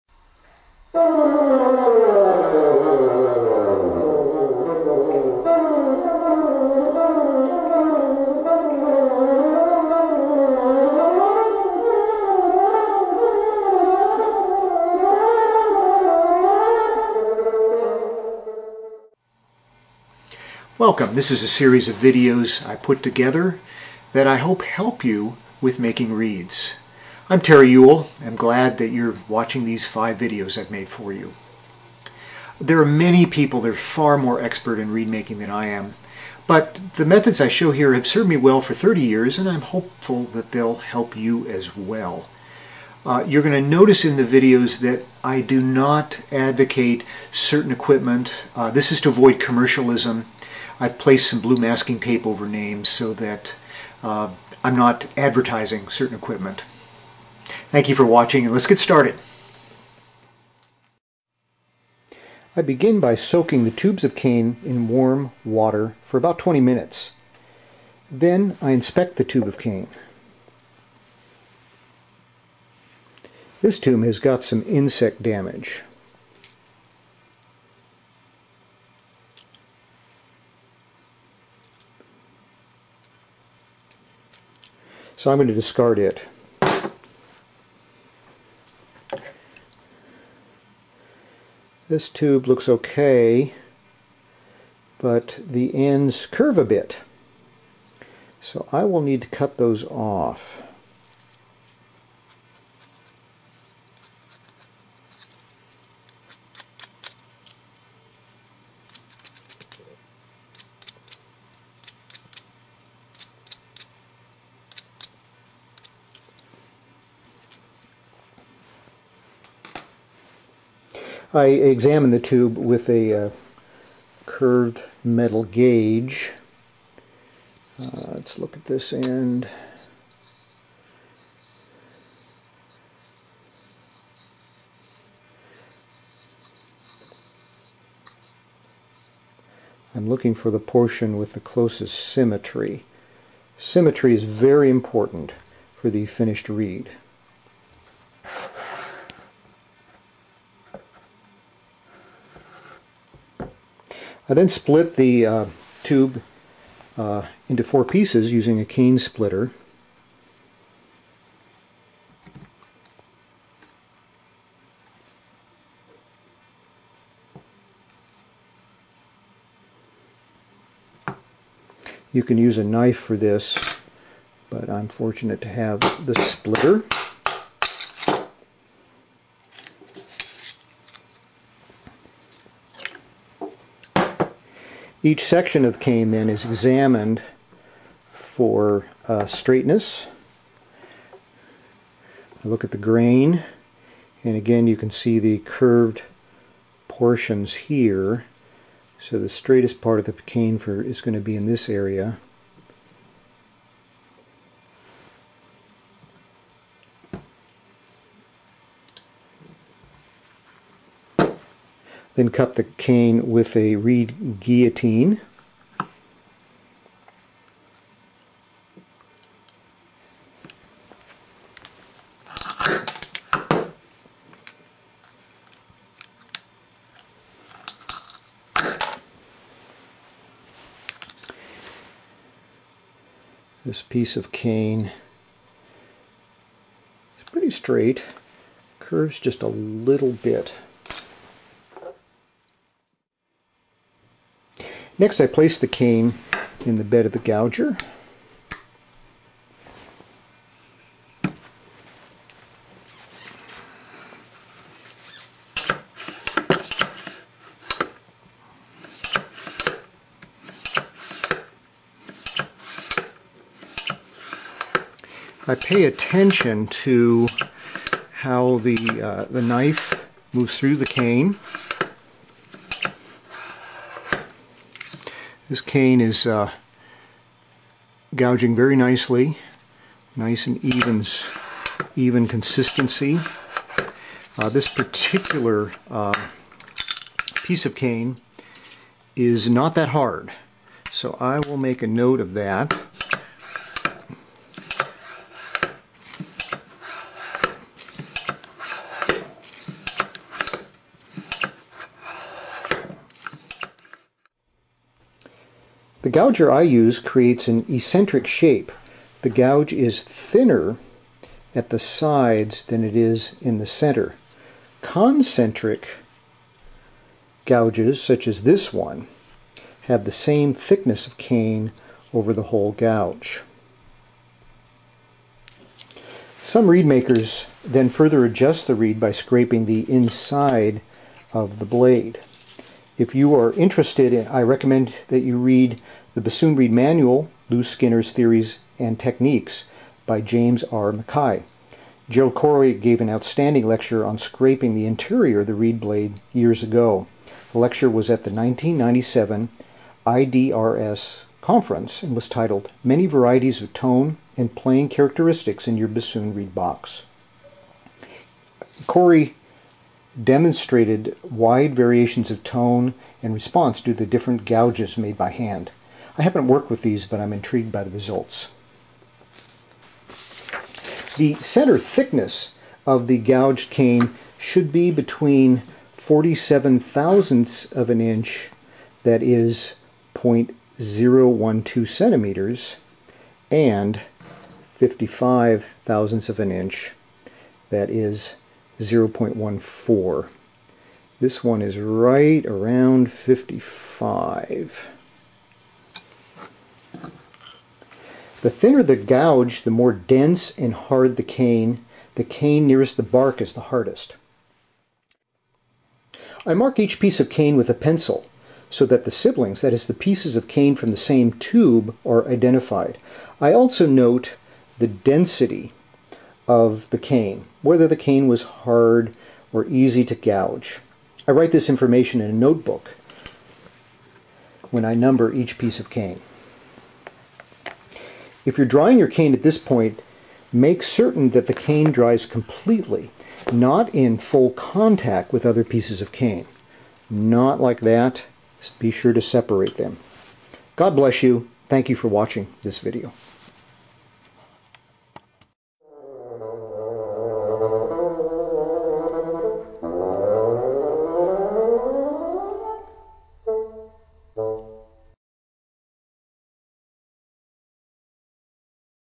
Bassoon Reed Making 1: Gouging
Terry B. Ewell demonstrates selecting, splitting, and gouging a tube of bassoon cane. Bassoon Digital Professor video #50.